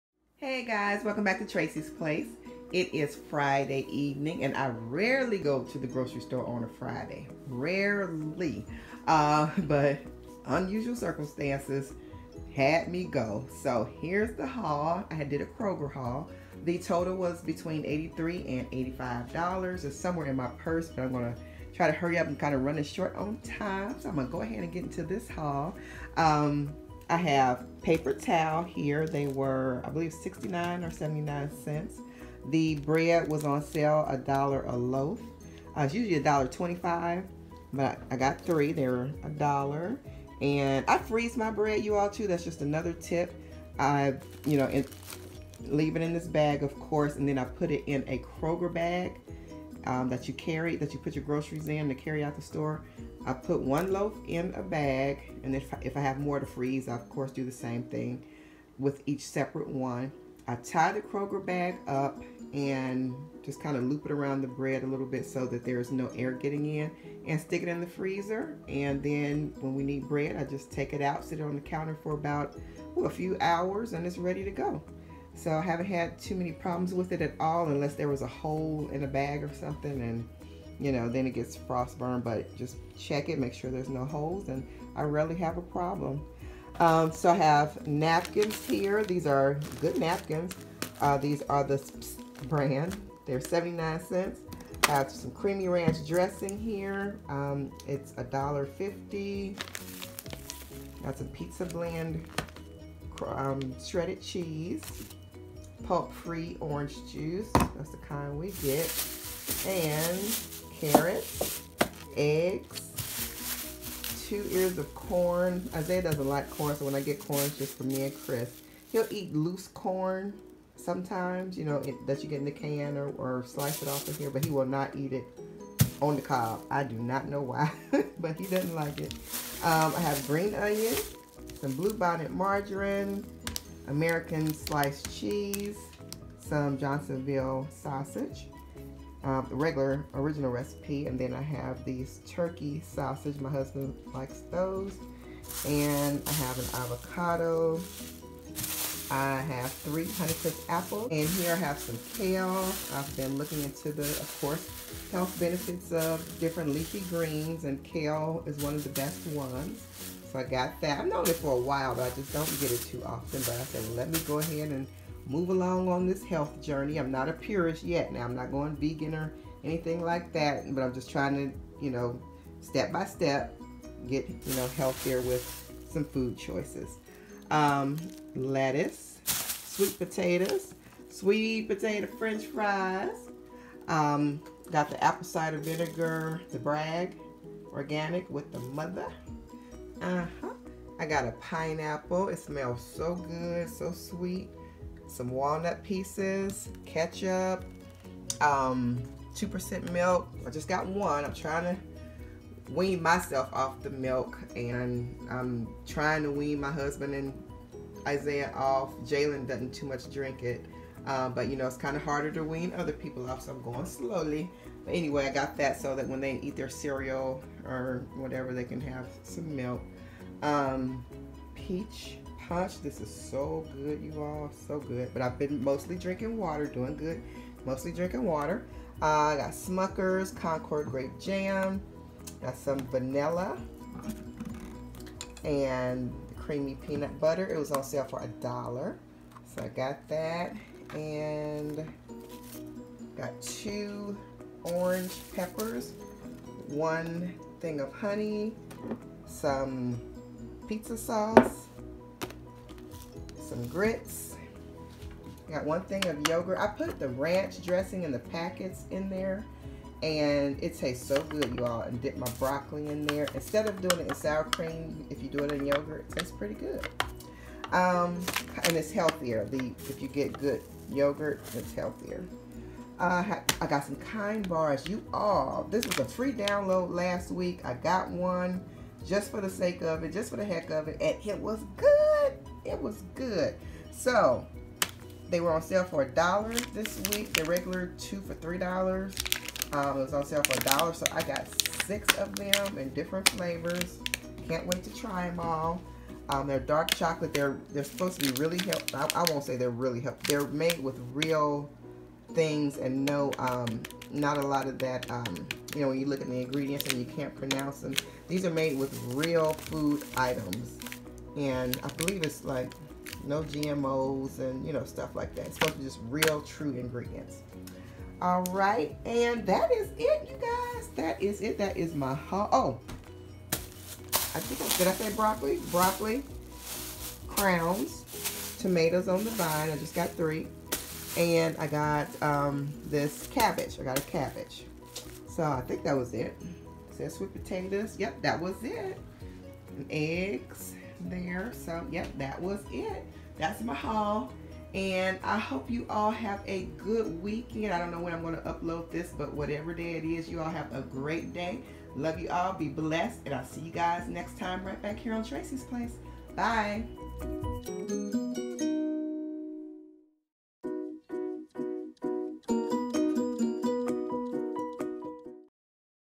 Weekly Kroger Haul 2019 | Grocery Haul
My Weekly Kroger Haul! Come along as I do my weekly grocery shopping!

To Send Me Mail :
Tracie’s Place
Cincinnati, Ohio
                  45240
To Send a Love Gift: